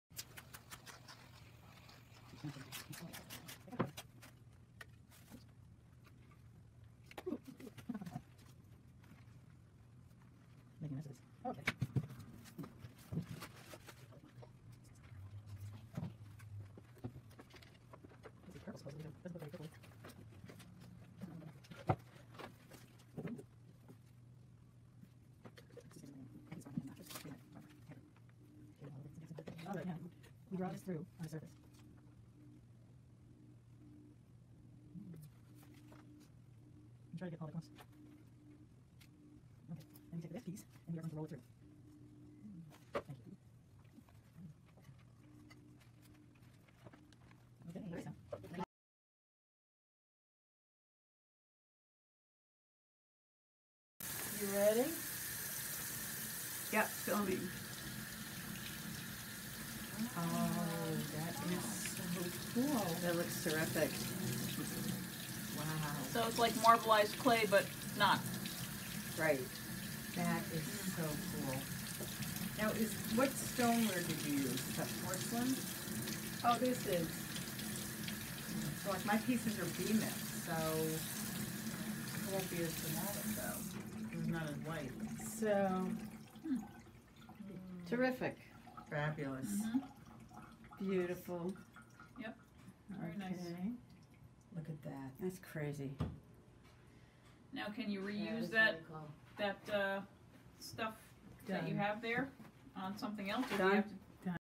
Underglaze marbling technique
Silvermine faculty member Clare Lewis  shares her technique of marbleizing underglaze using shaving cream.